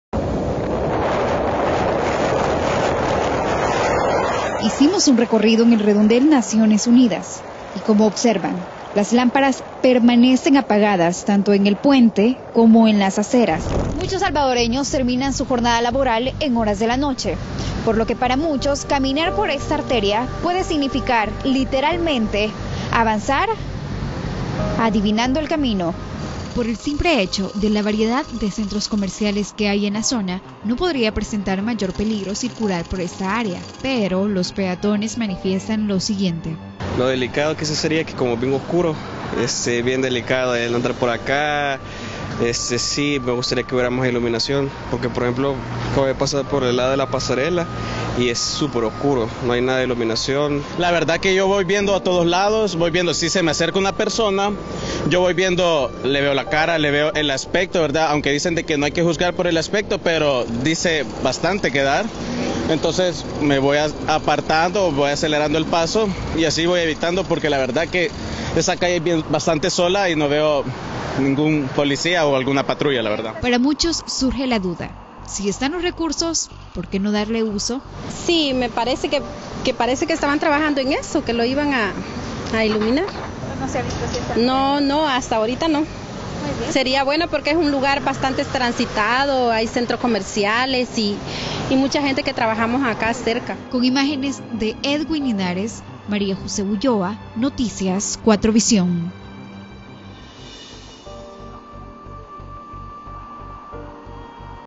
Zona redondel Naciones Unidas de noche.
Es una zona muy transitada y aunque parece un lugar seguro, quienes pasan a diario por ahí aseguran lo contrario. María José Ulloa nos explica.